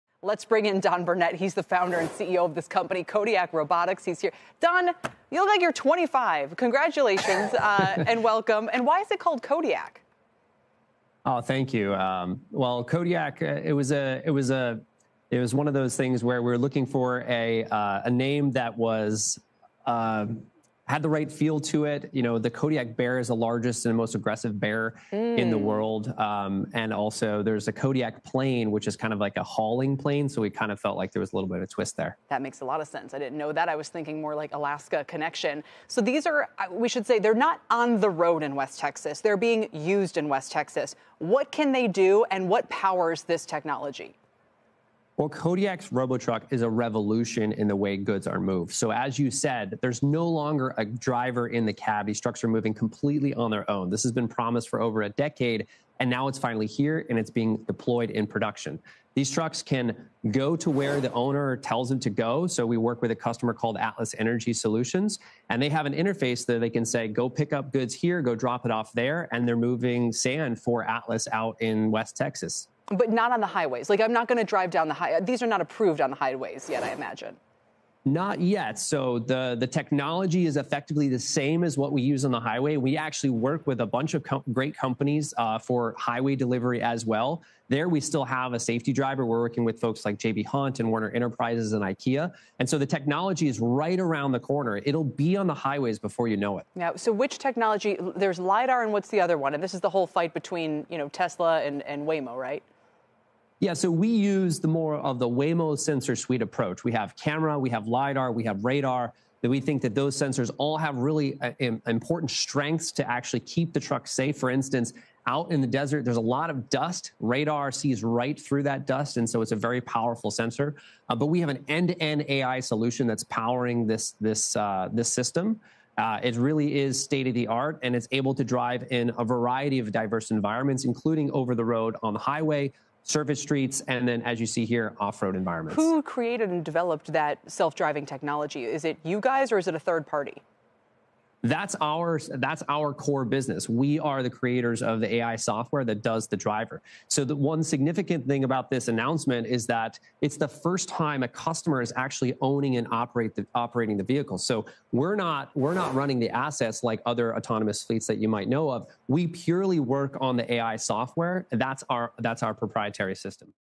Kodiak Robotics CEO on self-driving semi-trucks, launch to highways and technology
Don Burnette, Kodiak Robotics founder and CEO, joins 'The Exchange' to discuss his company's self-driving semi-trucks.